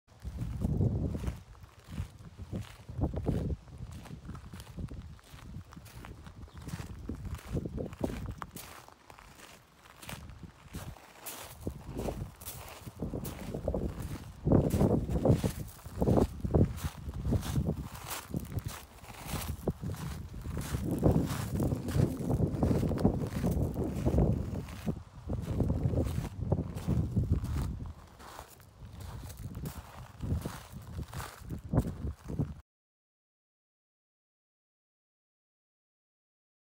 Köp Strängläggare Claas WS 327 på Klaravik
Strängläggare Claas WS 327
Fabrikat: Claas
Modell: WS 327
År: okänt
Chassienr / serienr: 37902559

Arbetsbredd: 3,27 meter

Begagnat skick